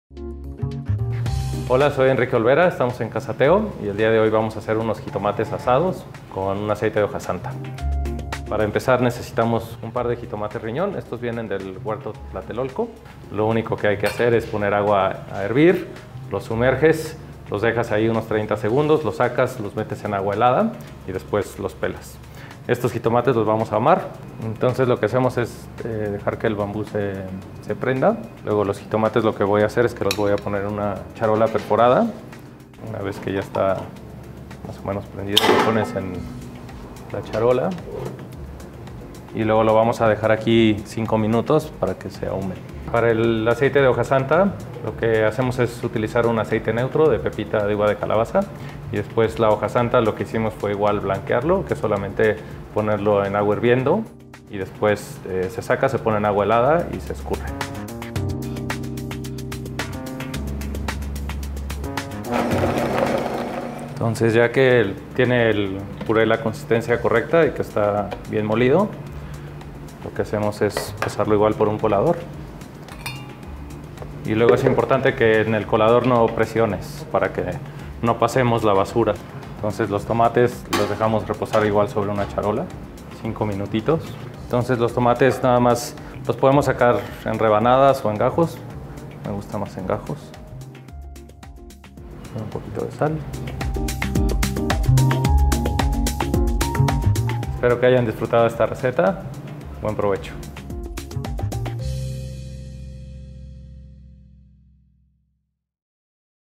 Amex Temp 2 Chef Enrique Olvera
Comercial para American Express donde conocemos un poco del gran Chef Enrique Olvera.